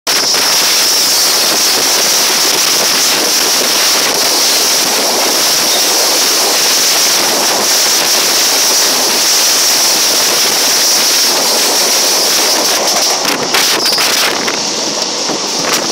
ERS WDG3A ERS-NZM Mangala Superfast in thrilling mood
Location:Idappally,8KM away from Ernakulam Jn and 11KM more to Aluva,Kerala.
Time:14th January 2014 13:36
17230 Hyderabad-Trivandrum Central Sabari Express meets ERS WDG3A 13222 hauled 21 coach 12617 Ernakulam Jn-Delhi Hazrat Nizamuddin Mangala Lakshadweep Superfast Express.

Departs Ernakulam at 13:20 daily and arrives Nizamuddin at 13:10 on 3rd day.
Inaugural Run Sat Aug 01, 1998
The train was earlier running between Mangalore and Delhi in round about route.Since the train was running till Mangalore, the name Mangala was derived from Mangaladevi, the most important deity of Mangalore city.
On February 22, 2013, 4 coaches of the 12618 Hazrat Nizamuddin-Ernakulam Mangala Lakshadweep Express derailed near Nashik, leavin 20 persons injured. On November 15, 2013, 10 coaches including all the AC coaches of the 12618 Hazrat Nizamuddin-Ernakulam Mangala Lakshadweep Express derailed near Nashik again at 06:40 a.m., leaving 5 persons dead and 50 injured. In both cases, track rupture is suggested as cause of the accident.